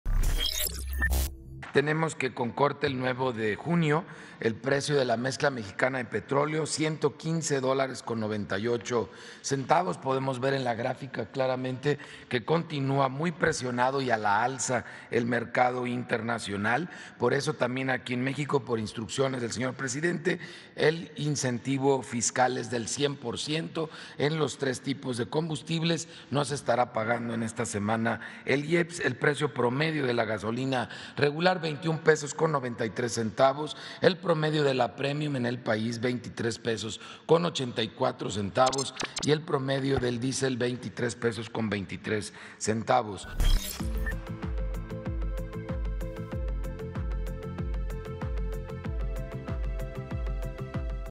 Esta semana no se pagará IEPS en gasolinas, gobierno los subsidiará al 100%: Profeco
Ricardo Sheffield Padilla, aseguró que el mercado internacional del petróleo “está muy presionado y a la alza”